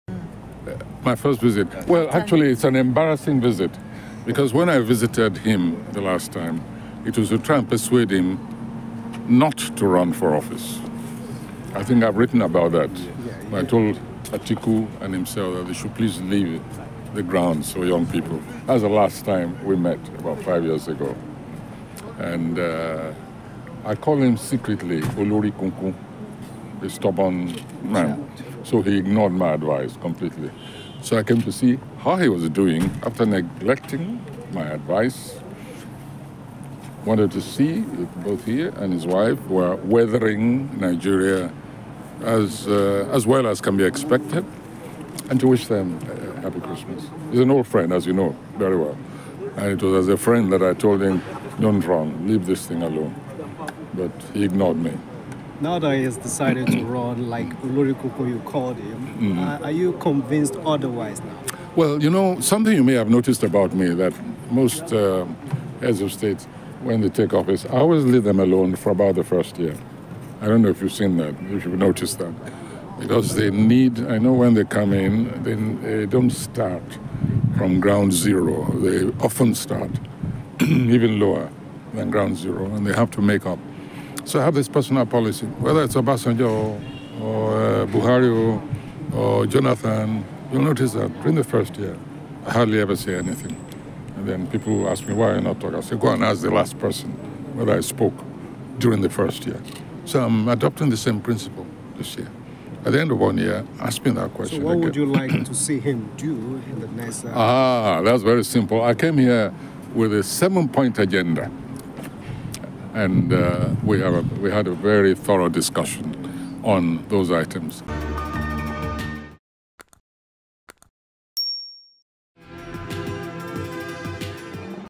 VIDEO: "He Is An Old Friend" - Wole Soyinka Warned Tinubu Against Running for Presidency
Nigerian Noble Laureate, Professor Wole Soyinka also visited the president whom he described as an old friend.

Prof. Soyinka says he had advised President Tinubu not to run for presidency but he turned down his advice.